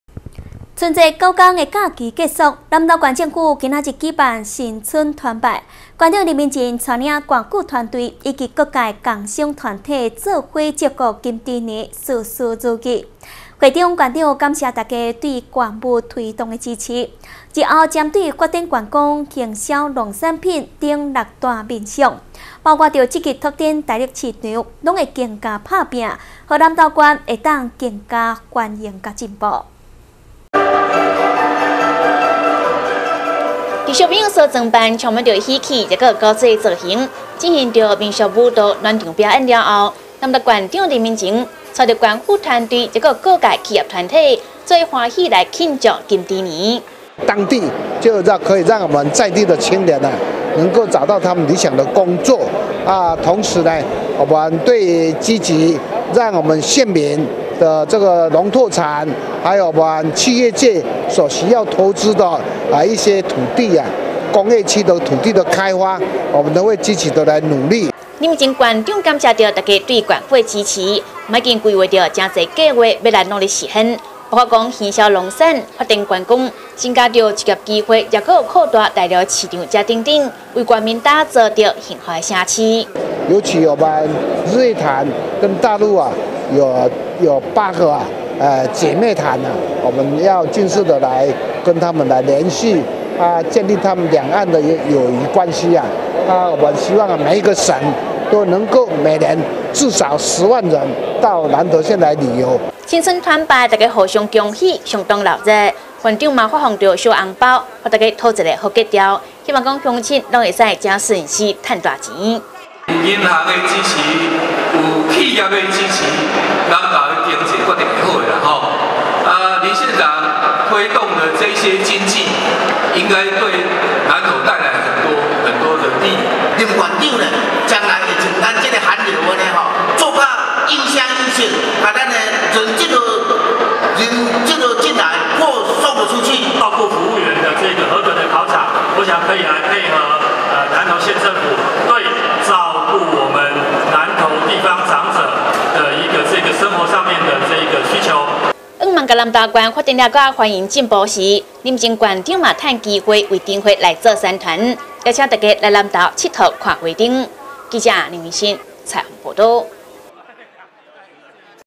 南投新聞 南投縣各界新春團拜
春節九天假期結束，南投縣政府今天舉辦新春團拜，縣長林明溱帶領縣府團隊以及各界工商團體，一起祝福金豬年諸事大吉，會中，縣長感謝大家對縣務推動的支持，日後針對發展觀光、行銷農產品等六個面向，包括積極開擴大陸市場，都會更加打拼，讓南投縣能更繁榮進步